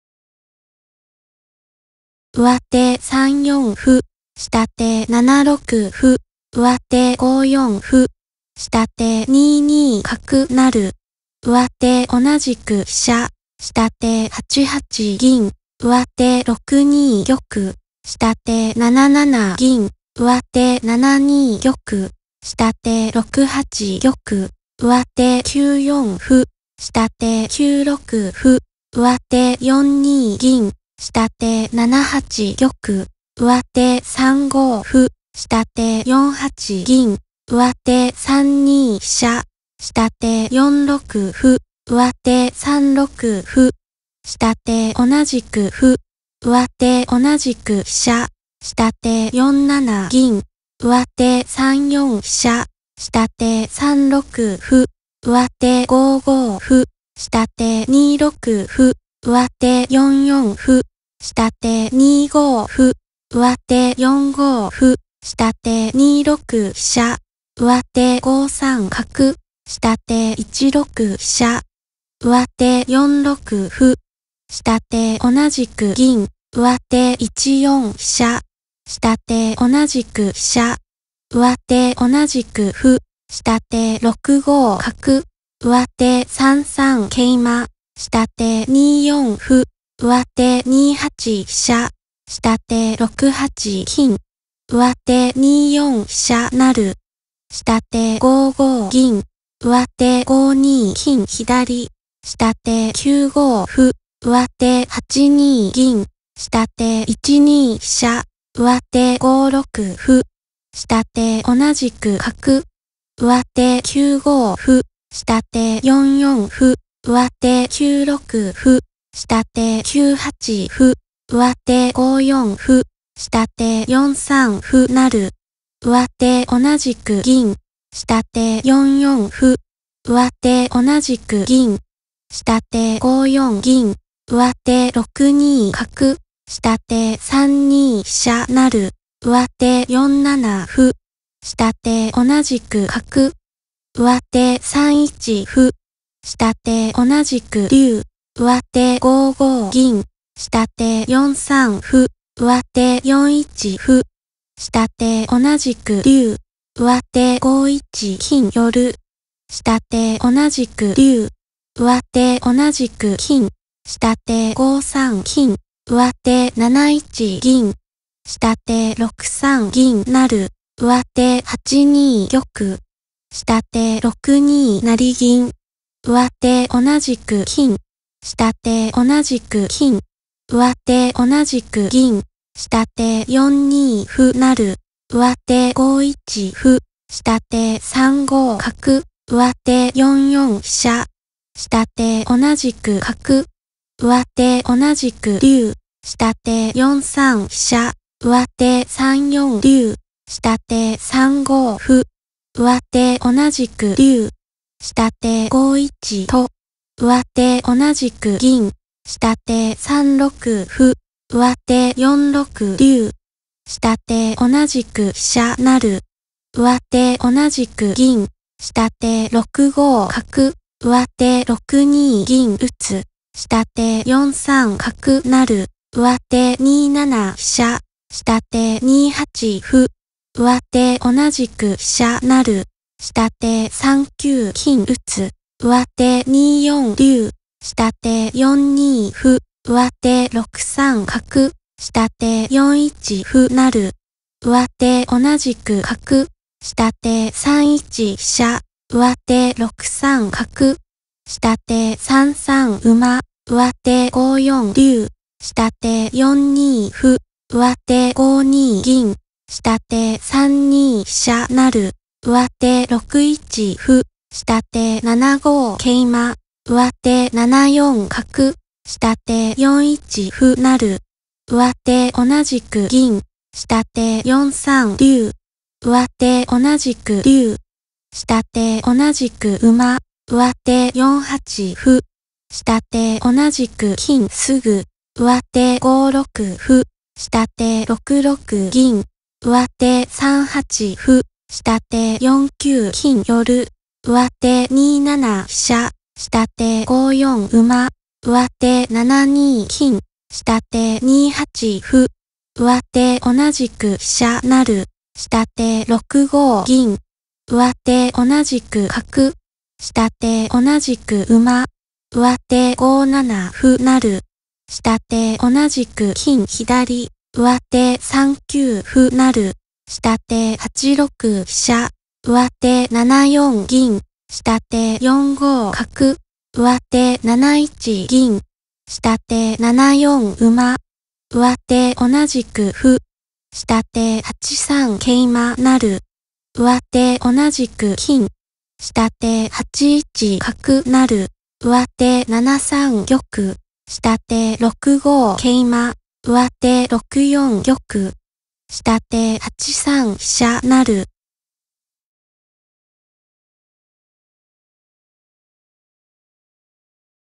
将棋 石本集 02 石本勾当VS伊藤看佐
文政八年六月二十六日　石本集二番　小日向新坂安藤氏
香落ち　石本勾当VS伊藤看佐
まで154手で下手 石本勾当の勝ち